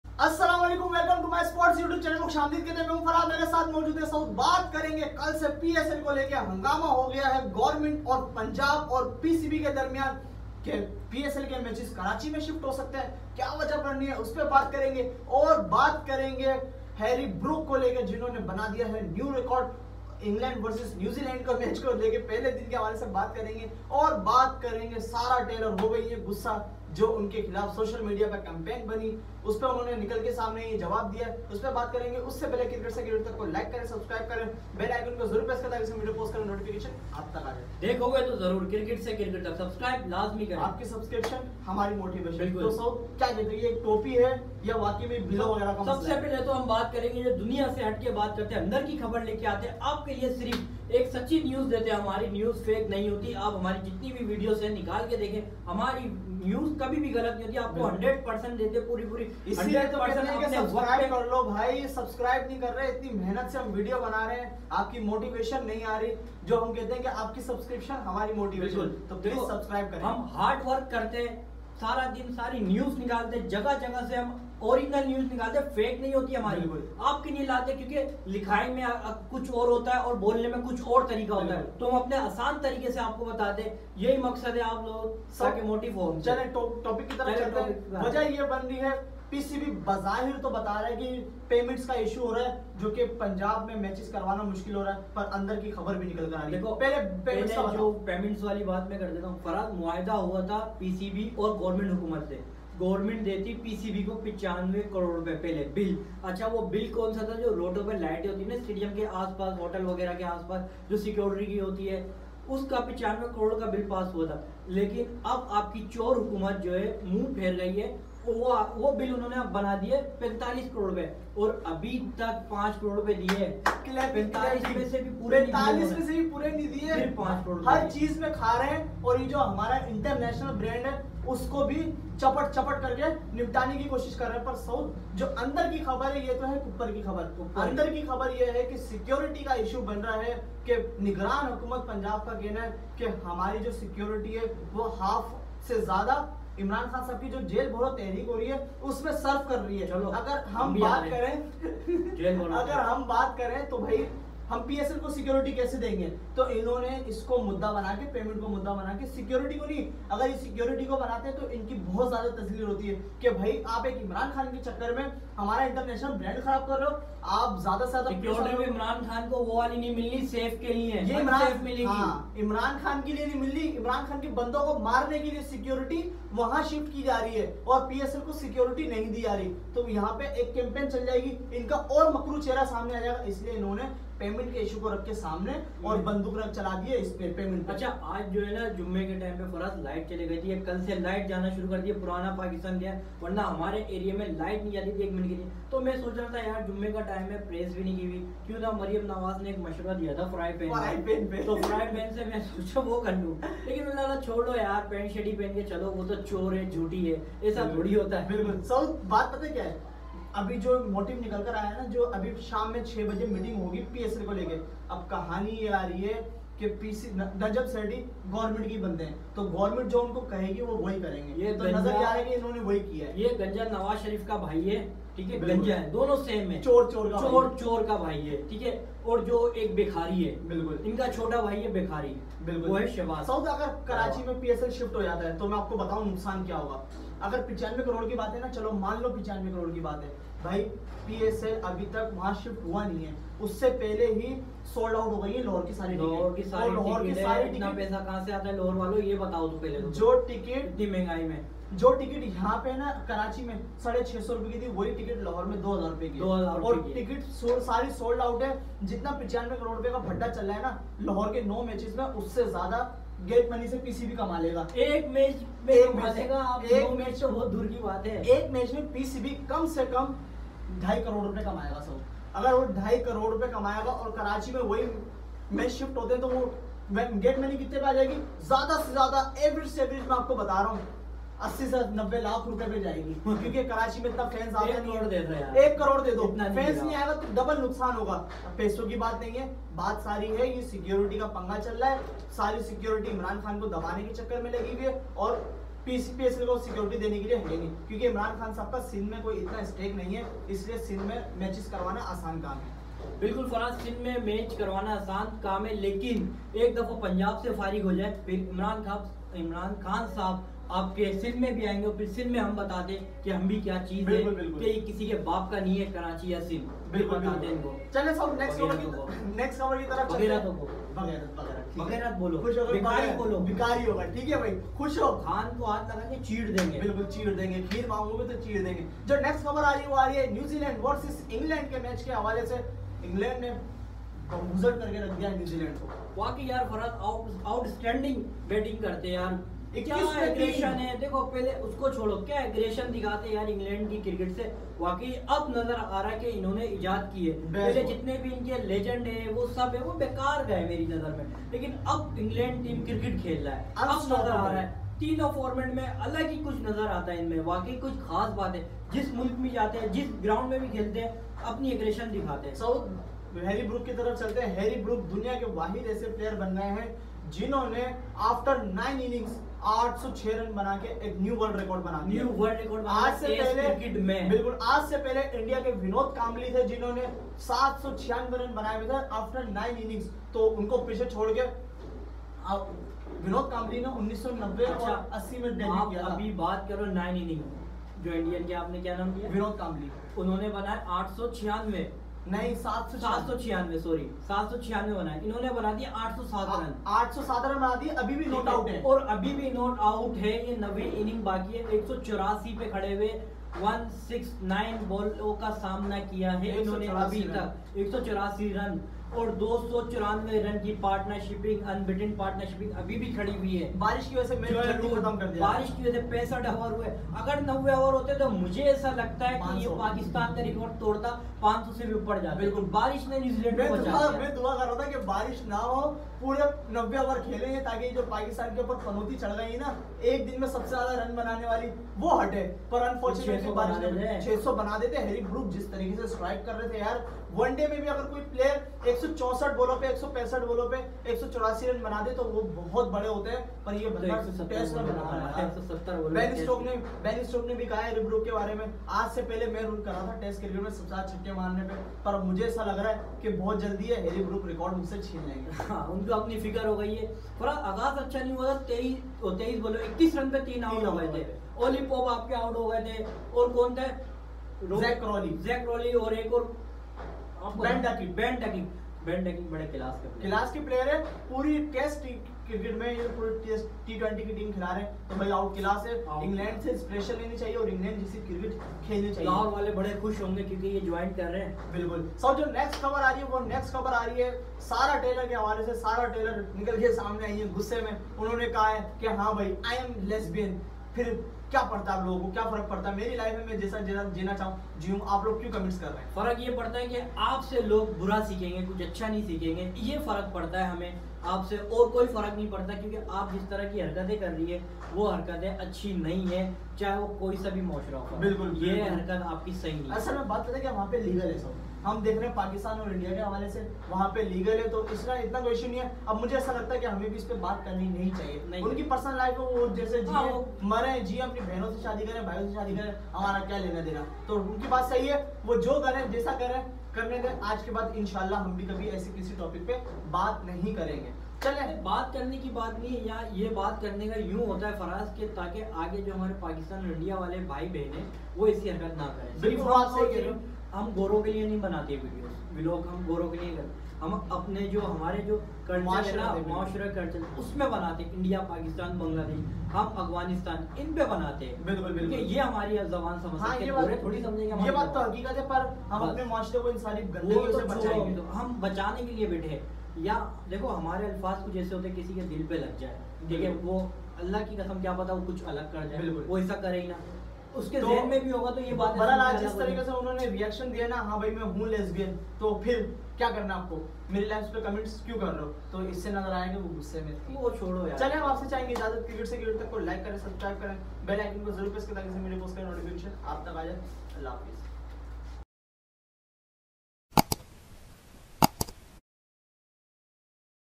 Bad news for PSL8 Harry Brook New world record Sarah Taylor Angry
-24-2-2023

Bad news for psl 8 shifted to Karachi harry Brook new world record Sarah Taylor angry